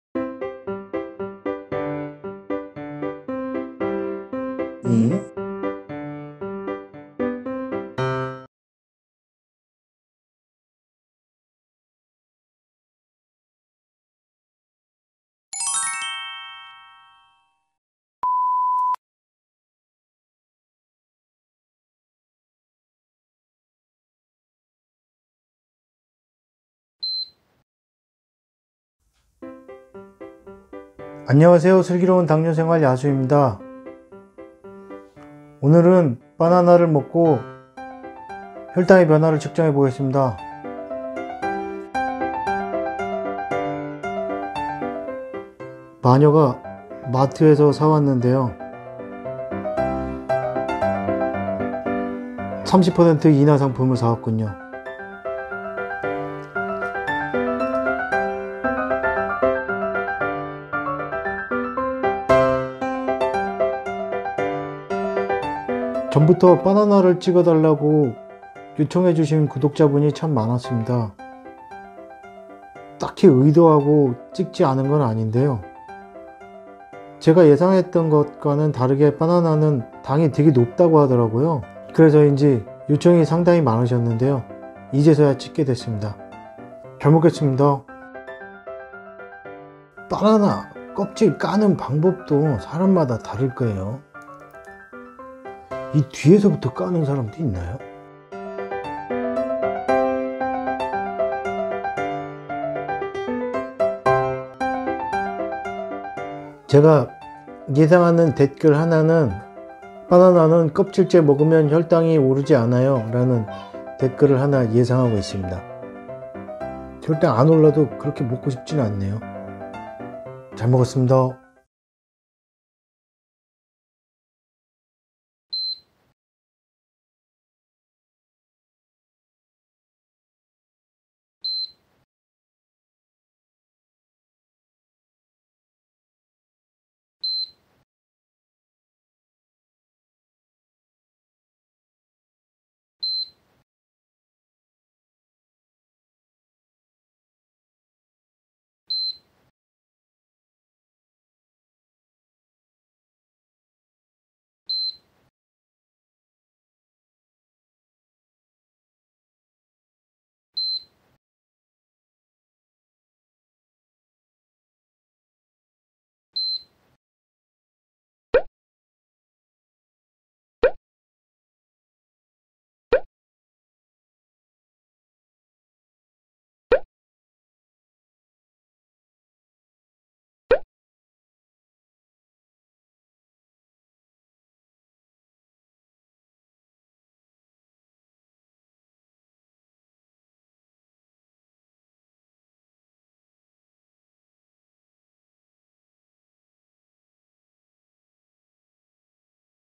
당뇨인이 바나나를 먹으면 혈당이 얼마나 오를까? (ft. 당뇨에 바나나가 나쁘다던데? |15분마다 혈당측정)
안녕하세요. 당화혈색소 11.9(공복 306)이었던 당뇨인 야수와 그의 아내 마녀가 이런저런 음식을 먹어보며 혈당을 체크하는 실험적 채널 "당뇨생활"입니다

오늘은 바나나를 먹고 혈당을 체크해보도록 하겠습니다 :)

2019년 3월 22일 당뇨약은 끊었고 고지혈증 약 복용중입니다